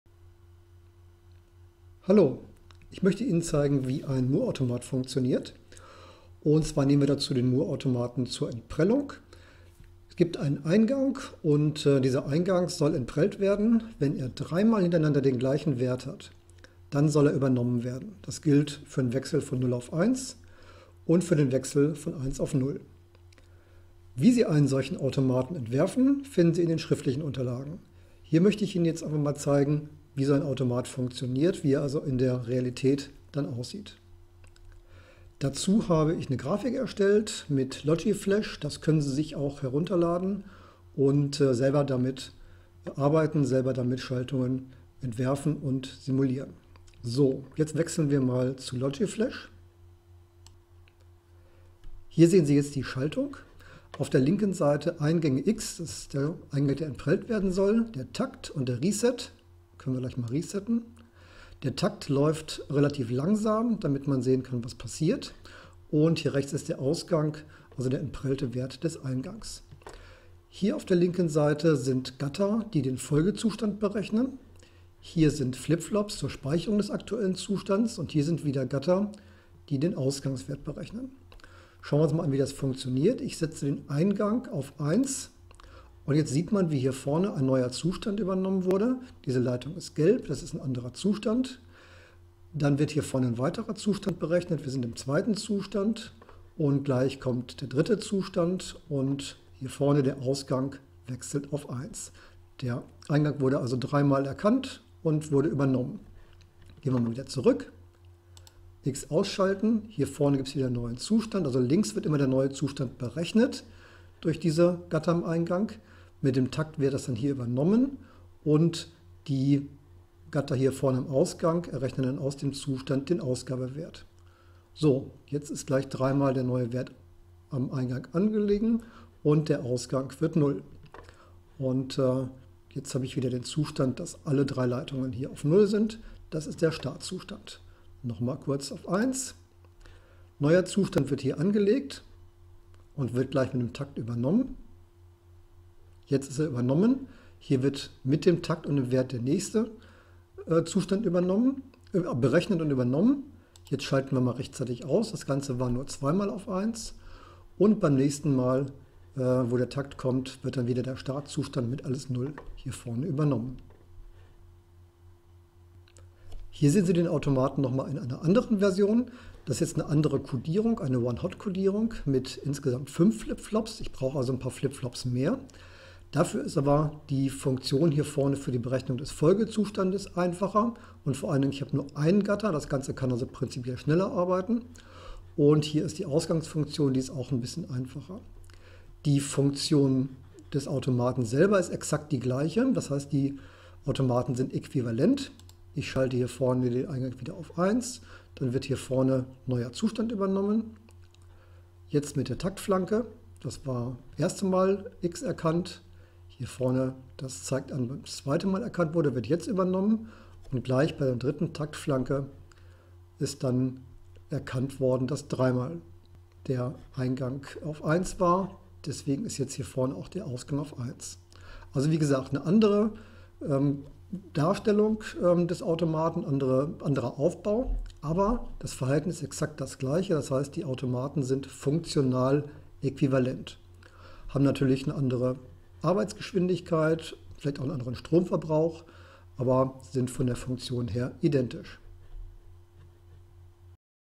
Digitaltechnik - Moore-Automat zur Entprellung
Animation eines Moore-Automaten zur Entprellung

Der Automat soll am Ausgang Y den entprellten Wert des Eingangs X angeben. Wenn am Eingang drei Takte lang der gleiche Wert ‚0' oder ‚1' anliegt, soll der Ausgang Y diesen Wert annehmen. Ansonsten soll der letzte Eingangswert, der mindestens drei Takte anlag ausgegeben werden.